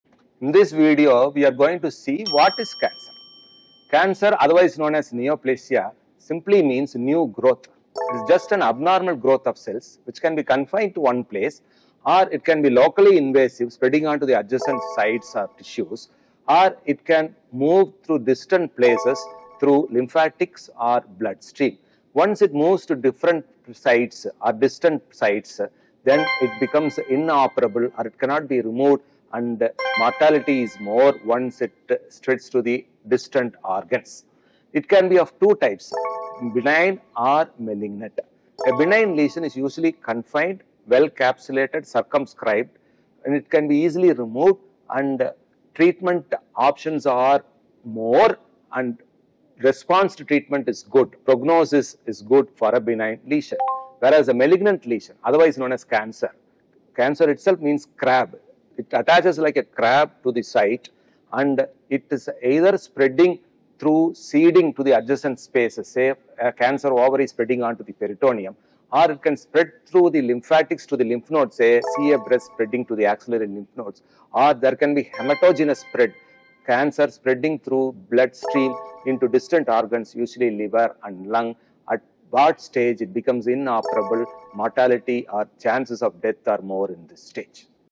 What is Cancer? How Cancer Spreads in the Body | Dr. P. Murugesan Explains!
In this short video, Dr. P. Murugesan breaks down what cancer (neoplasia) is and explains how it spreads within the body. Learn the basics of this complex disease and stay informed about your health. Dr. P. Murugesan dives into the process of cancer metastasis and its impact on the human body. Learn about the causes, mechanisms, and how early detection plays a crucial role. Stay informed and take charge of your health!

Dr. P Murugesan (Consultant Diabetologist),
Coimbatore, India